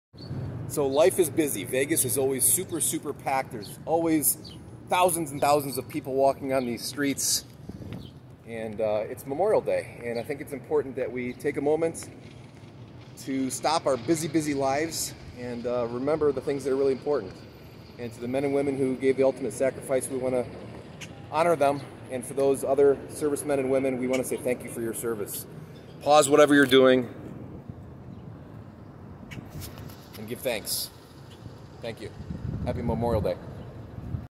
A very quiet Las Vegas Strip on Memorial Day 2020
A quiet Las Vegas on Memorial Day 2020.